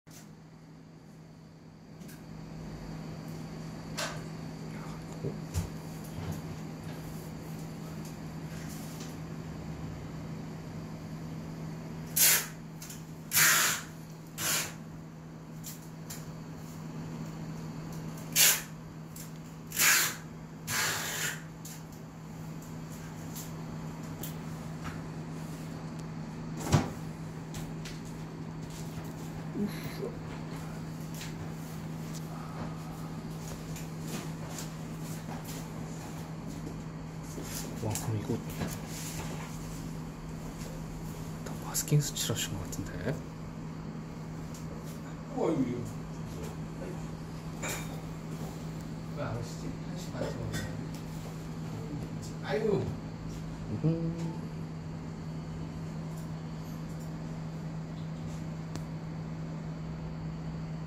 튜닝타임즈 프라모델 강좌 AK플라자문화센터 분당점 함병설님의 Evolution 에볼루션 1/100 RGM-79S GM Spartan 짐 스파르탄 2 완성
AK플라자문화센터 분당점 프라모델 수업 수강생 함병설님이 레진 컨버전 키트를 반다이 MG 키트에 적용시킨 짐 스파르탄 2 를 완성하셨다고 보여 주셨는데요, 하나하나 마스킹하고 색칠해서 데칼까지 깔끔하게 붙여 마무리를 해주셨습니다^^ 튜닝타임즈